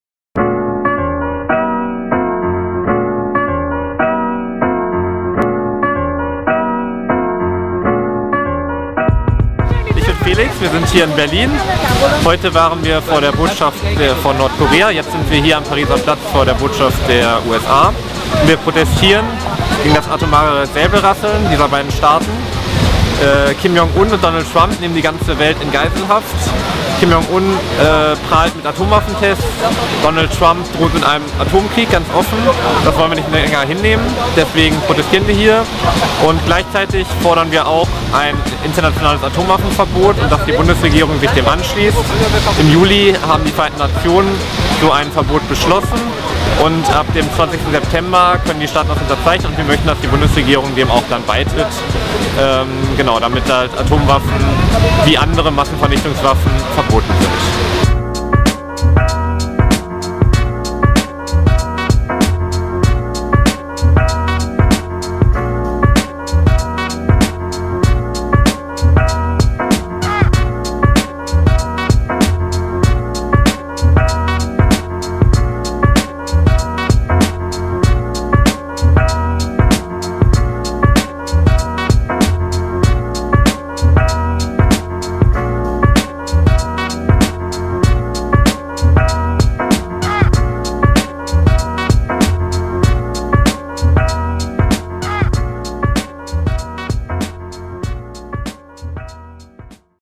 USA vs. NORDKOREA: Protest gegen atomares Säbelrasseln!
KIM JONG-UN, DONALD TRUMP: BEENDET DAS ATOMARE SÄBELRASSELN! Wenn es zu einem Krieg kommt, verlieren ALLE! Gemeinsam mit der "Internationalen Kampagne zur Abschaffung von Atomwaffen" (ICAN) und den "Ärzten zur Verhütung eines Atomkriegs" (IPPNW)​ haben wir Mitte September 2017 vor der nordkoreanischen- und der US-Botschaft gegen das atomare Säbelrasseln beider Staaten protestiert! Mehr Informationen dazu gibt's im Video.

Hilf' mit solche und andere Aktionen gegen Krieg und Aufrüstung zu realisieren